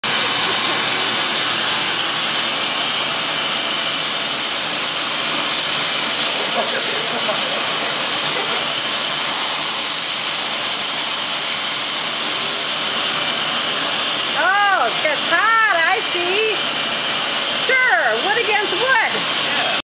Burnishing the edge of the bowl
Nice contrast to the spalted maple. The kids got to keep this one.